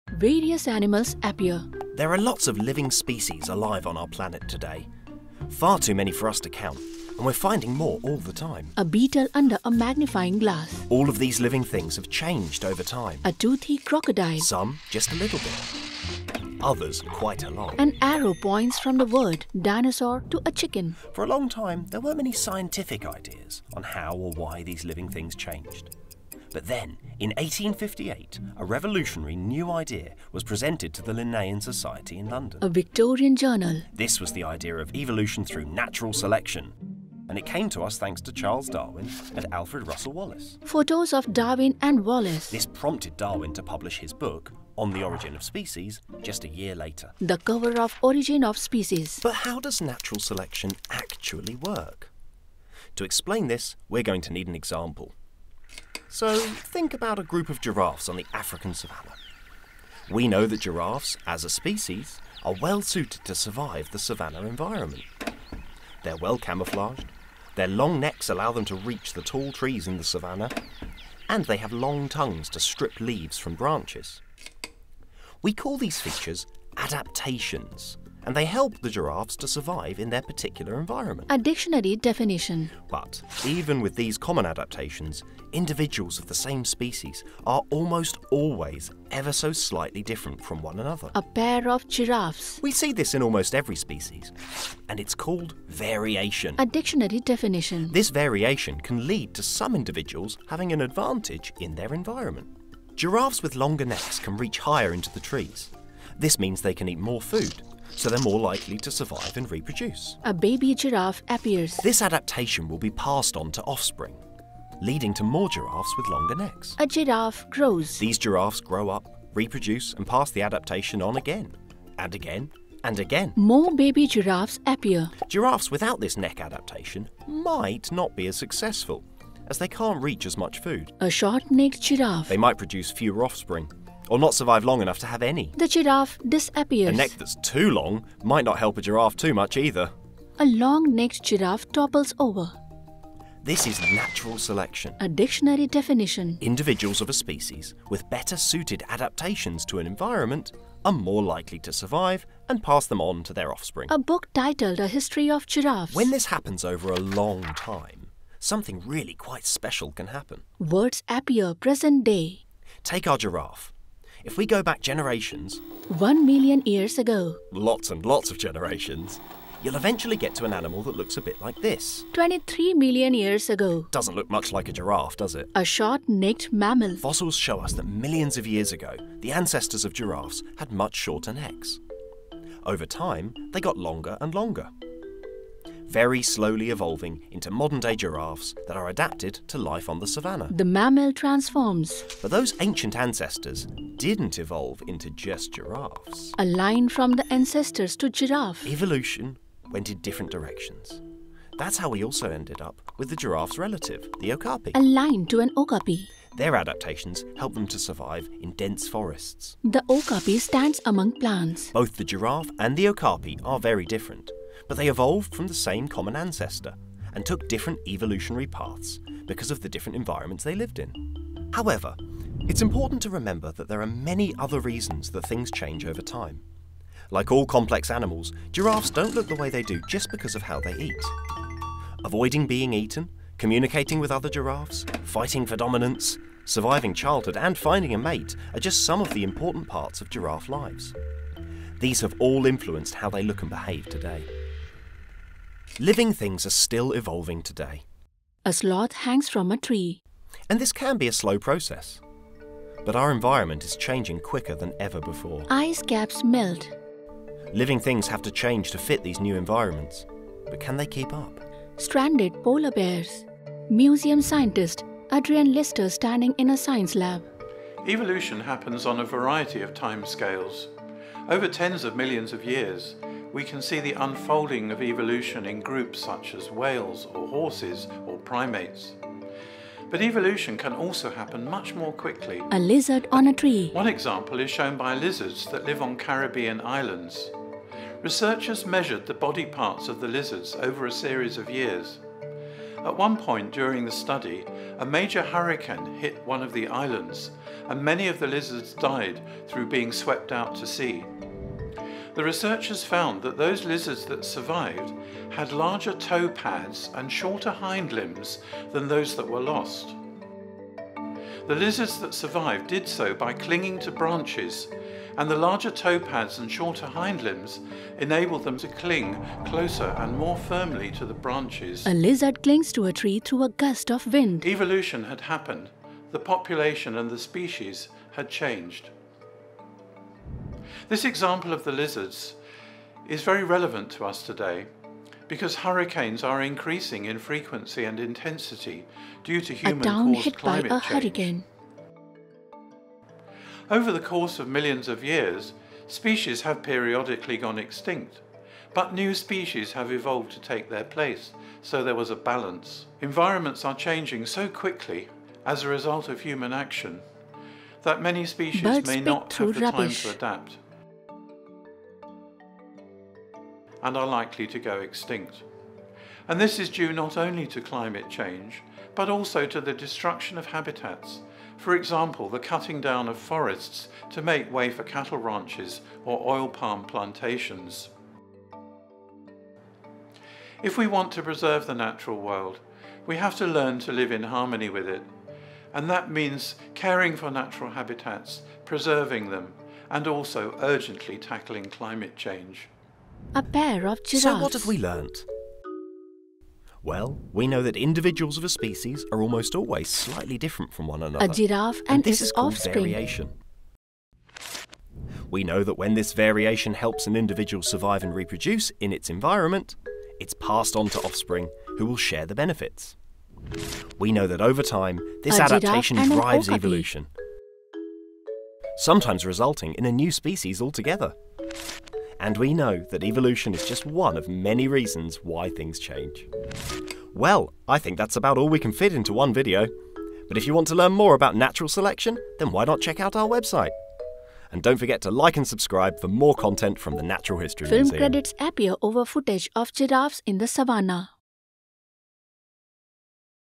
What is natural selection? | Natural History Museum (Audio Described)
AUDIO DESCRIBED

From adaptation to evolution, we explore the theory of natural selection.

Dive into one of Charles Darwin's most famous ideas to see how we account for the millions of species on Earth and learn why living things change over time.

00:00 Introduction
00:17 Where does the idea of natural selection come from?
00:44 How does natural selection work?
02:27 How does evolution work?
03:28 Other reasons why living things change?
03:51 Are things still evolving today?
05:27 Does climate change affect evolution?
06:05 Summary of natural selection